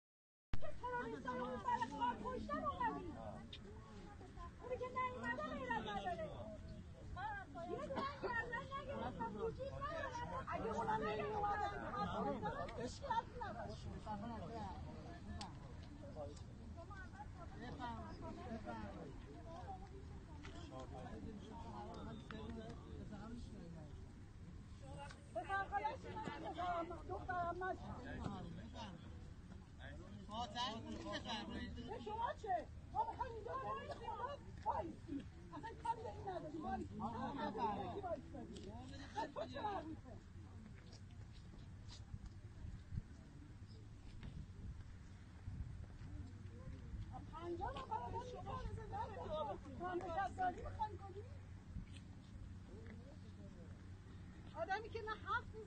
وحوش خامنه ای مردم را حتی از ایستادن در کنار مزار مهدی حضرتی منع میکنند
مراسم چهلم مهدی حضرتی

وحوش خامنه ای مردم را حتی از ایستادن در کنار مزار مهدی حضرتی منع میکنند که یک بانوی شجاع به یکی از مزدوران میگوید : به شما چه ربطی داره که ما چرا اینجا ایستاده ایم .
و چرا اینجا آمده ام  اونی که نیامده غیرت نداره  و کسانی که و جود نداشتن نیامده اند
   وحوش خامنه ای اجازه‌ی حضور مردم را نداده و سعی در بیرون کردن مردم از «بهشت علی» دارند